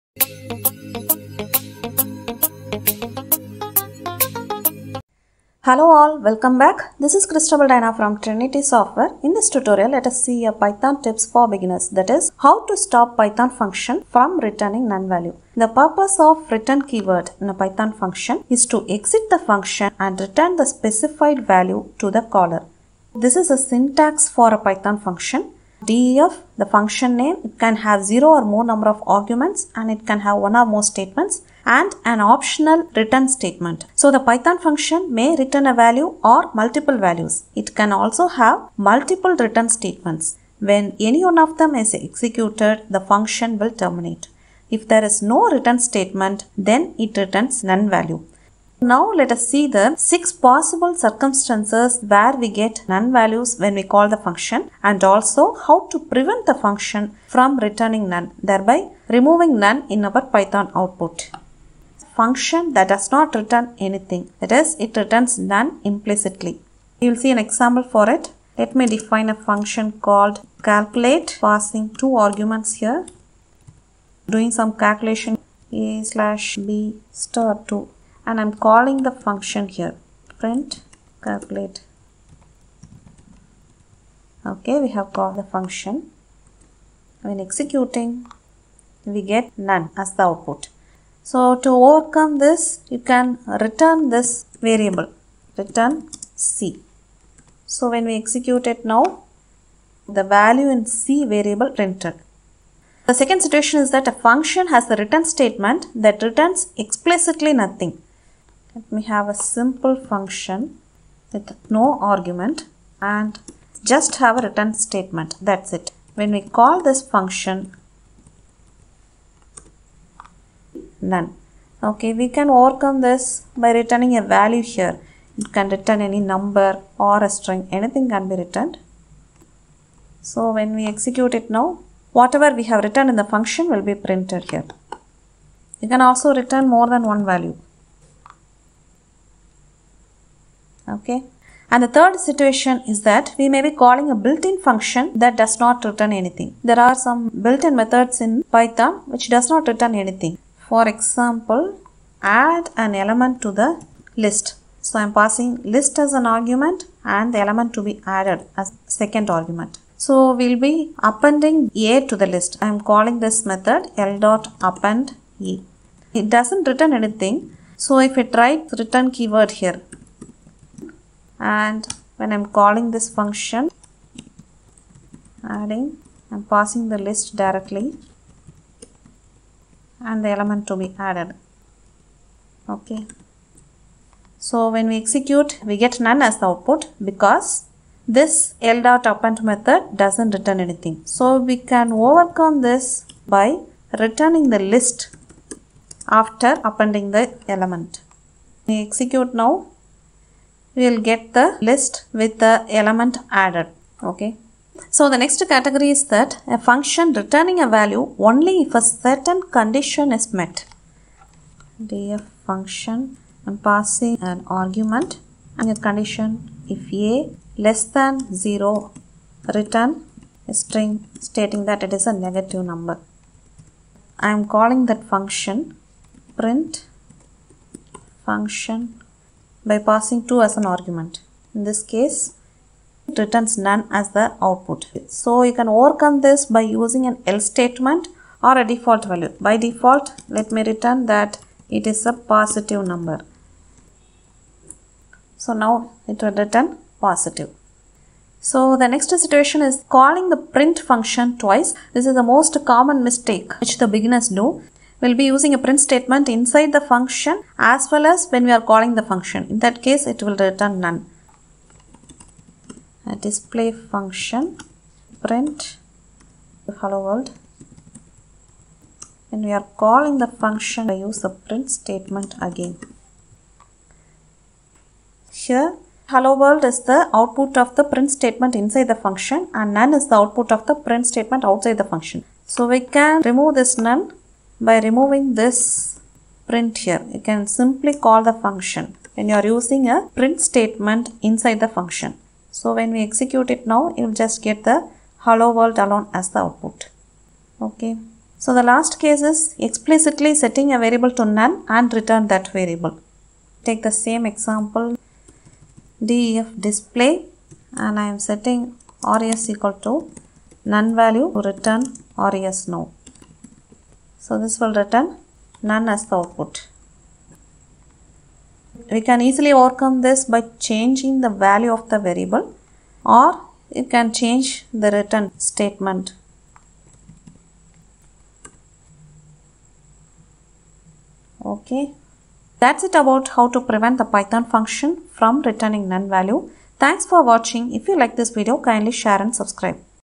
How to stop Python function from returning None
This tutorial explains about various situations where a Python function returns None as output. And also examples show, how to stop python function from returning None value.